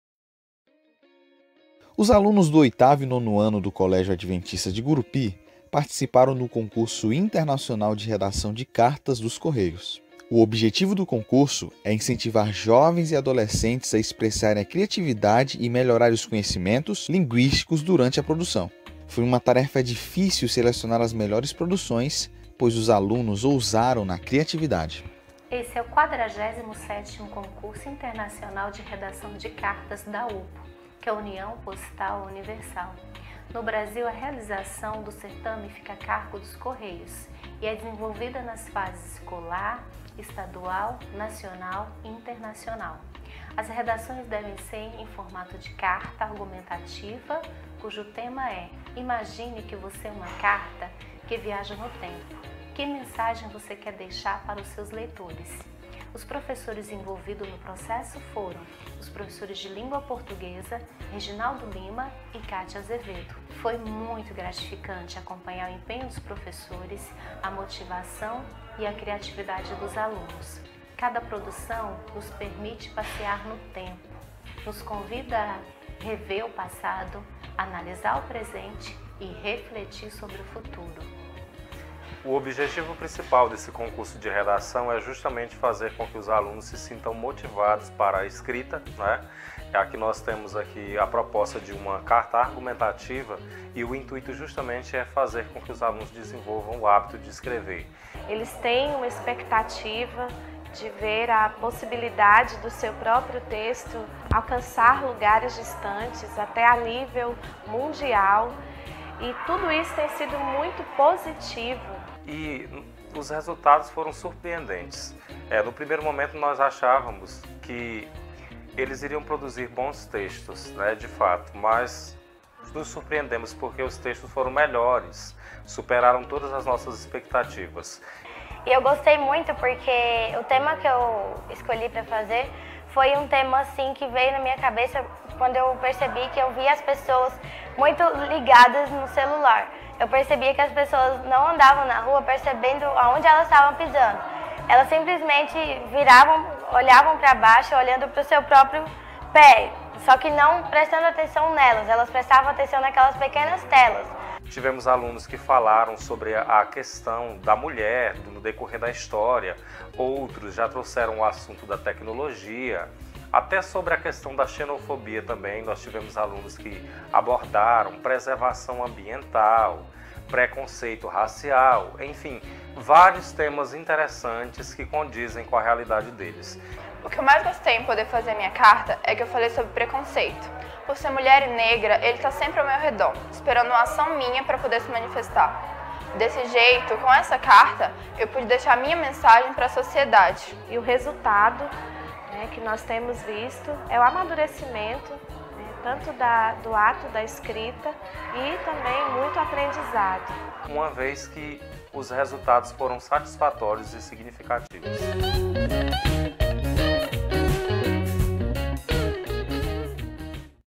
47º Concurso Internacional de Redação de Cartas
Com o tema “Imagine que você é uma carta que viaja no tempo. Que mensagem você quer deixar para seus leitores?”. O objetivo do concurso é incentivar as crianças e adolescentes a expressarem a criatividade e a melhorarem os conhecimentos linguísticos. Os professores envolvidos no processo foram os professores de Língua Portuguesa: Reginaldo Lima e Kátia Azevedo.